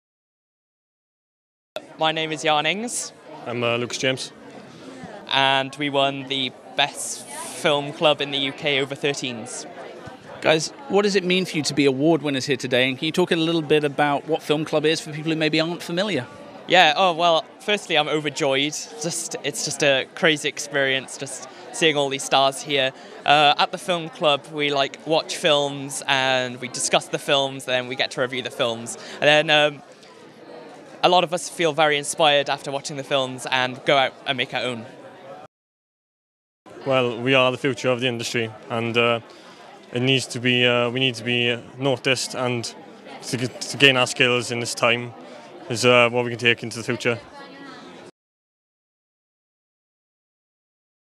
Film Club Of The Year Interview Into Film Awards 2018
Red Carpet News TV talks to winners and VIP guests at the Into Film Awards 2018 in London at the BFI Southbank. VIP Interviews include George MacKay, Susan Wokoma, Craig Roberts, Katie Leung, Georgina Campbell, Fionn O’Shea, Rosie Day, Hermione Corfield, Tomiwa Edun, Gemma Cairney, Rhianna Dhillon, Justine Simons and Tom Davis.